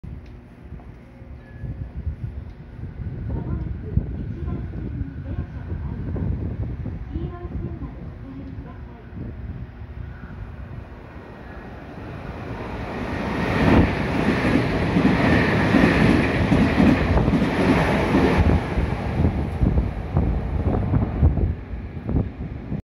●20211219 2200系 通過 ＠山王７
撮影路線：名鉄 名古屋本線
撮影駅：山王
行先：特急 名鉄岐阜
車両：2200系
両数：6両
撮影機材：【スマホ】SONY Xperia XZ2 Premium
MH：なし
備考欄：